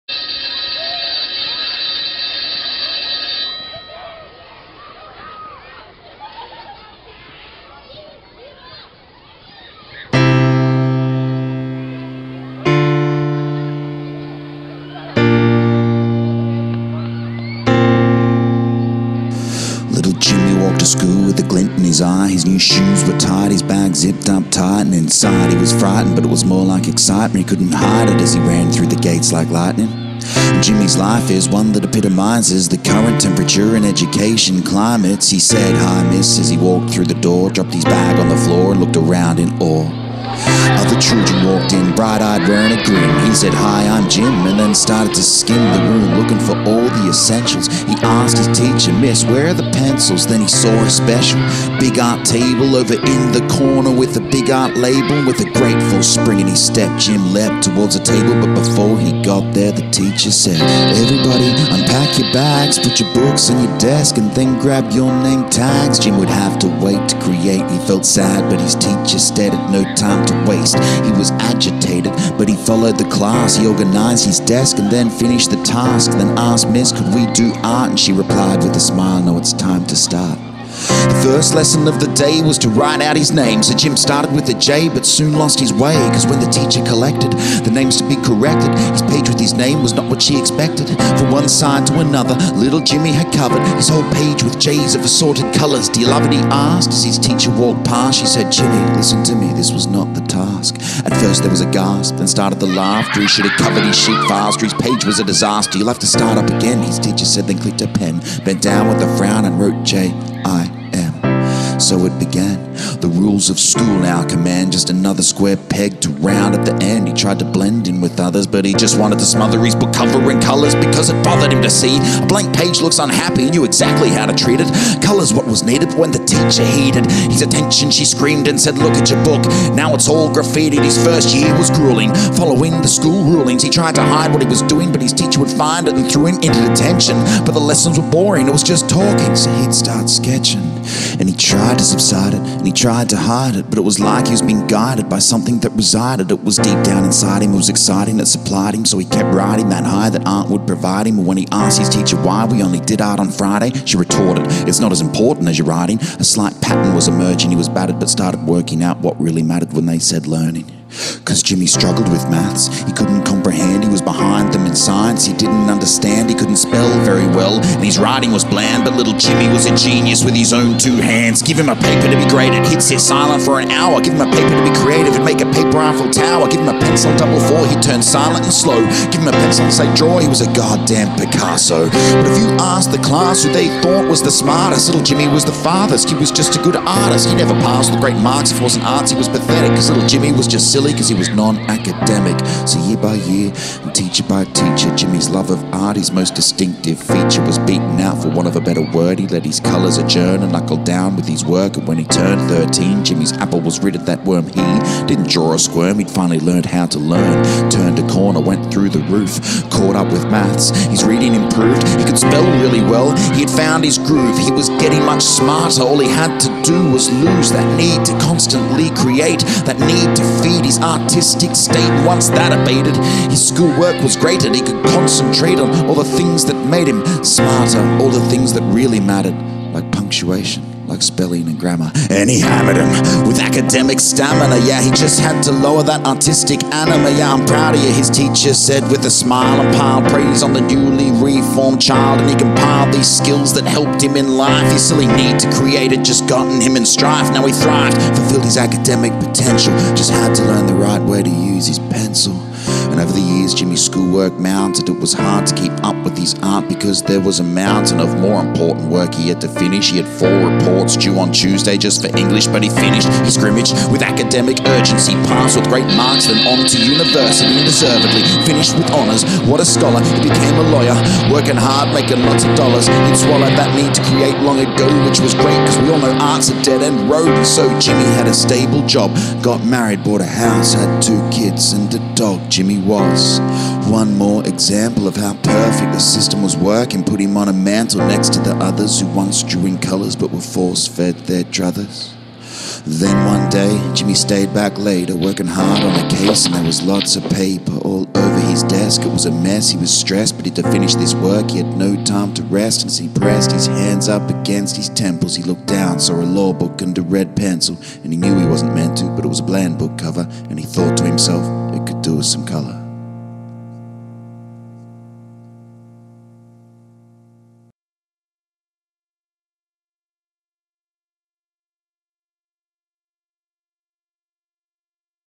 Little Jimmy
A thing I did ages ago...
Very heightened, but creativity really is squashed in schools.
If you don't like it... Go watch Jimmy 1.5 (Below)